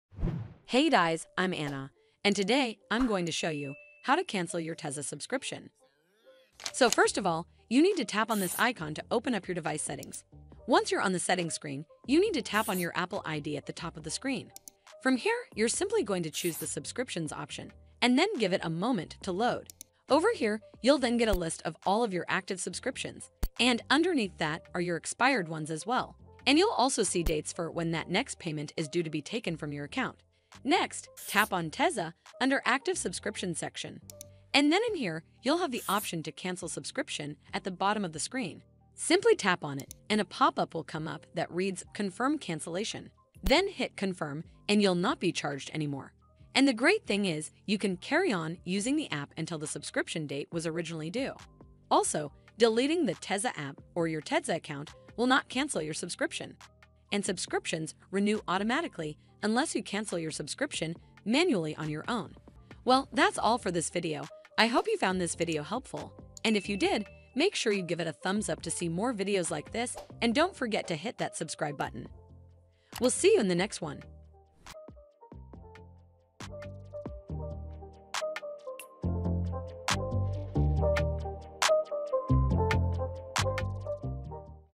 How to Cancel Tezza Subscription
In this video, I'll show you How to Cancel Tezza Subscription. This is the easiest and fastest way to Cancel Tezza Subscription. Make sure you watch until the end of this video to find out How to Cancel Tezza App Subscription on ios. These methods work on iOS 11 as well as iOS 12, iOS 13, iOS 14, iOS 15 and iOS 16. Hope you enjoy!

Video Parts:
00:00 Intro: How to Cancel Tezza Subscription
00:07 Canceling Tezza App Subscription
01:01 Outro: Ending